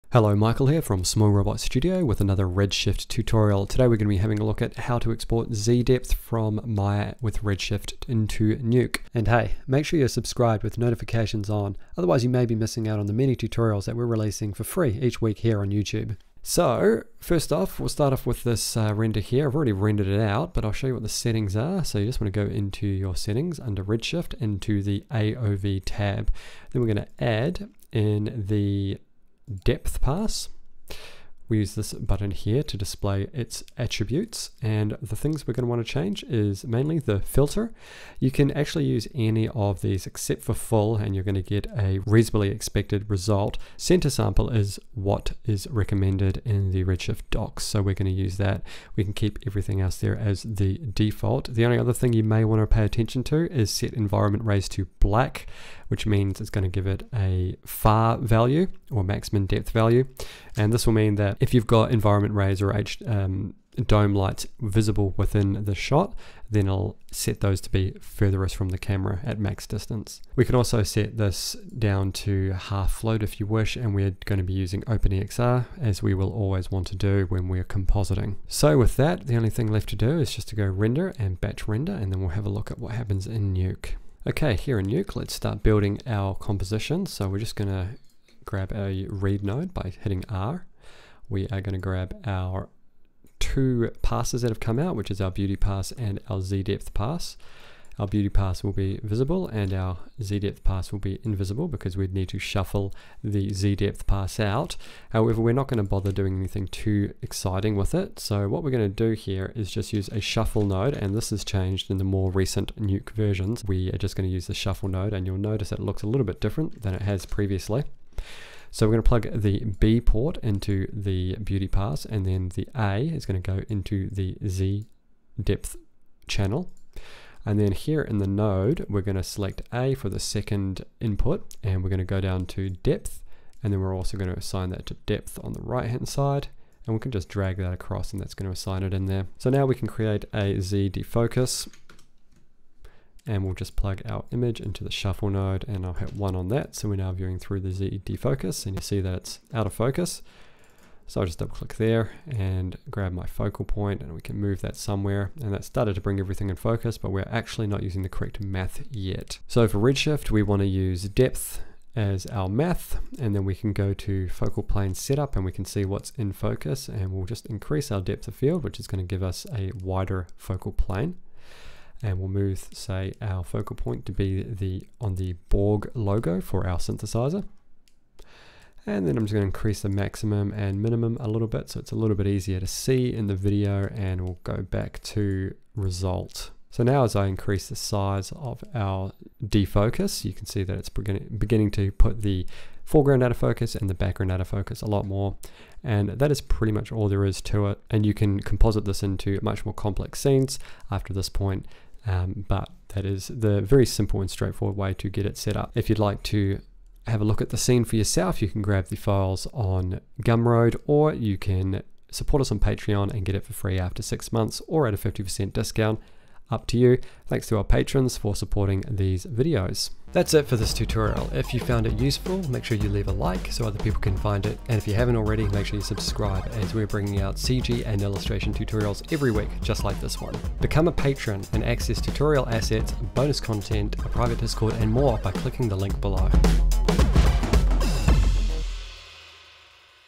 Redshift Z Depth with Nuke Tutorial
In this tutorial we'll set up a zDepth AOV pass for Nuke with Redshift.